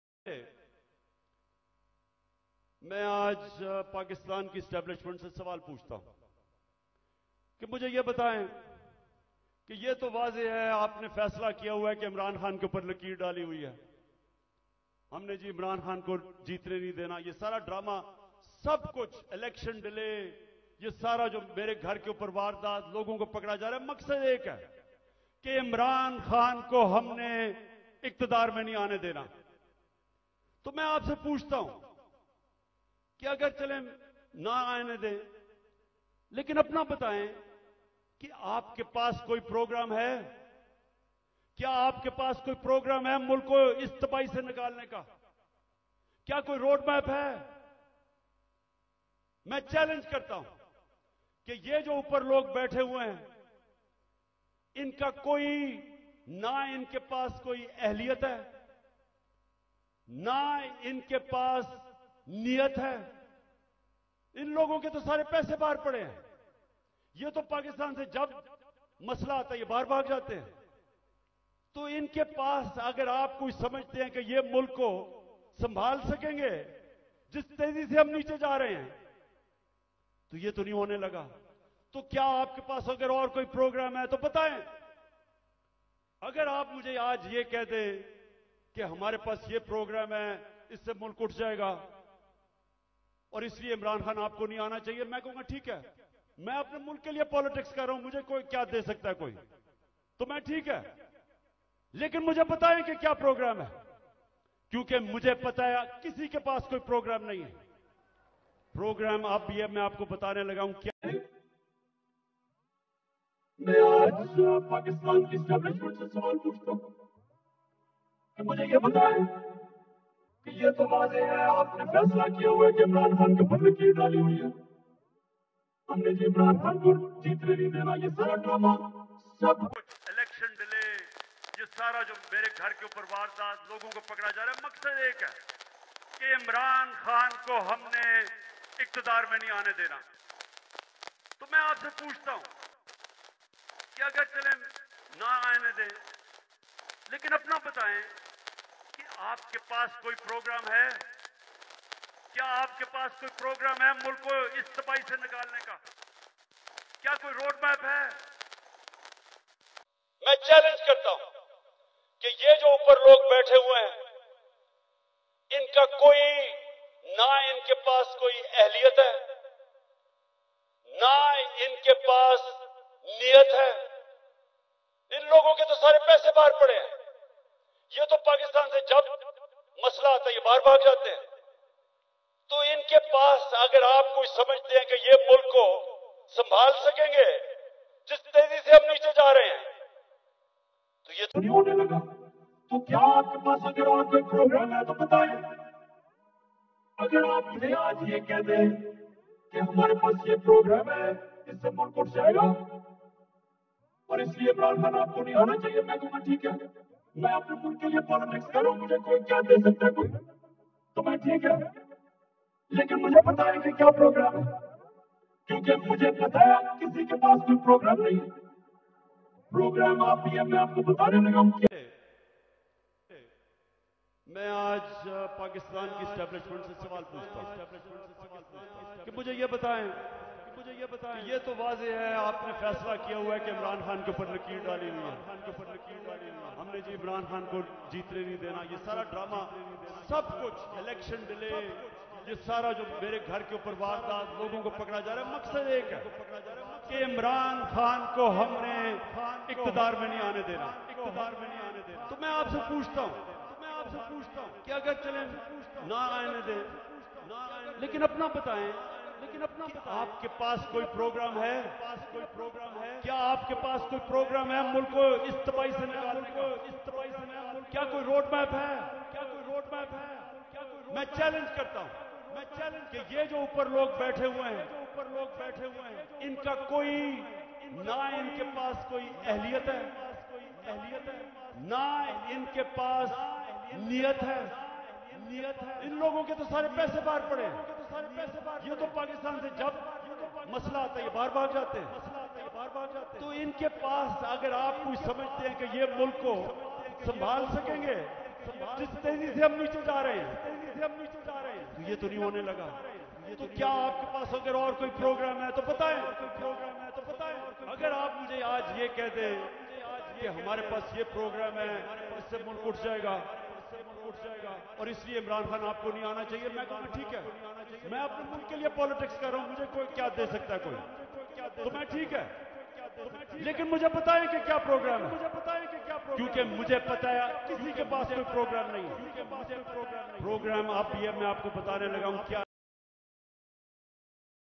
عمران خان نے چیلنج کر دیا PDM اور Establishment کو۔
عمران خان نے چیلنج کاری دیا PDM اور Establishment کو۔
Imran Khan challenge Nawaz and team for Program to stable Pakistan economy۔